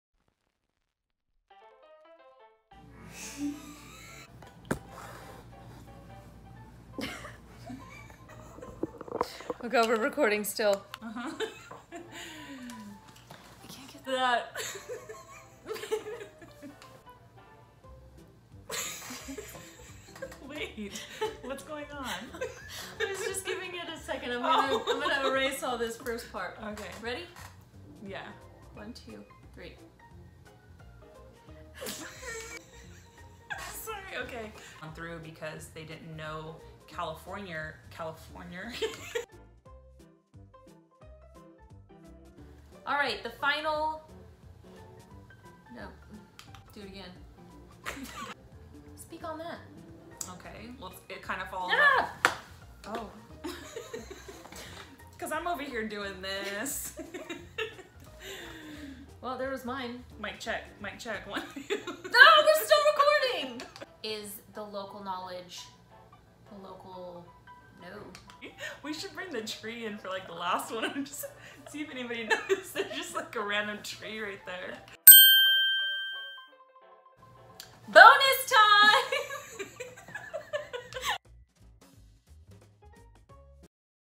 BLOOPERS - Top 5 Reasons to Choose a Local Lender
If you can’t tell, Rickelle and I have a pretty good time together.

Casey Budge
BRE 02074940
Century 21 The United Group

Rickelle Stockton
NMLS 1729345
Maciel Mortgage